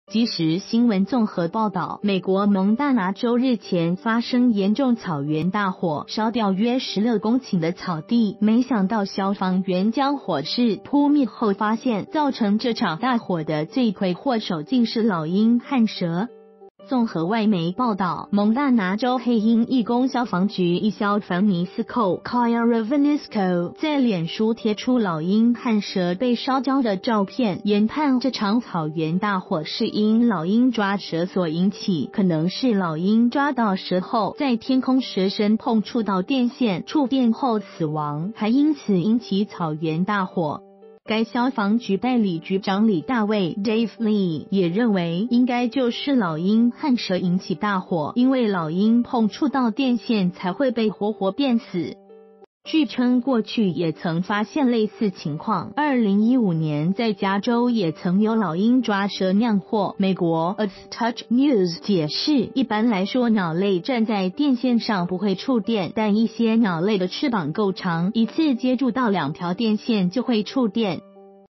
美國草原大火延燒16公頃 揪出禍首竟是他們...
〔即時新聞／綜合報導〕美國蒙大拿州日前發生嚴重草原大火，燒掉約16公頃的草地，沒想到消防員將火勢撲滅後發現，造成這場大火的罪魁禍首竟是老鷹和蛇！

發表評論評論如下。